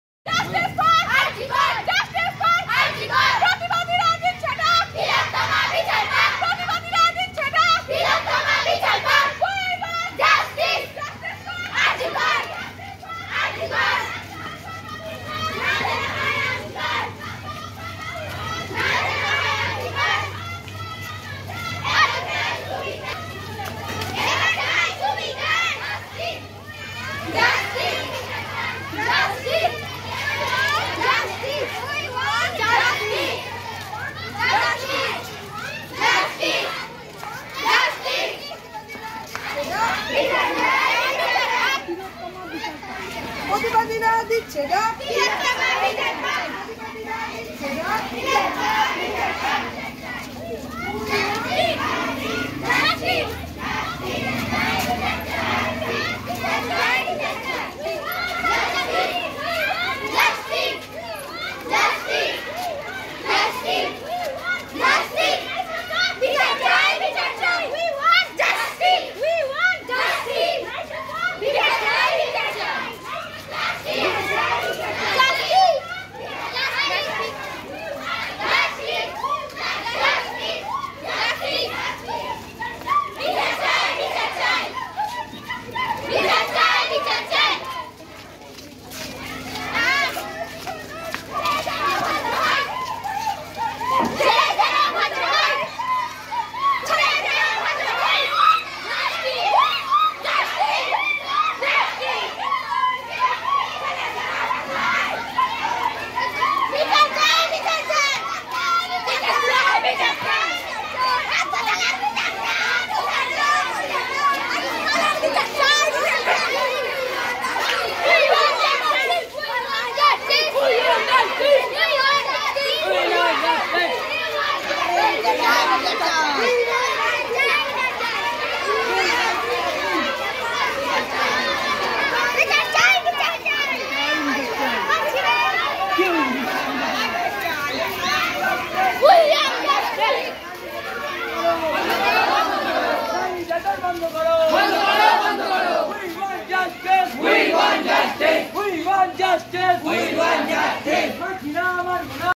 Protest | Hatkalna Gajalaxmi High School| Kolkata Horror
Protest for Justice by School Students. Kolkata R.G Kar Medical College Horror.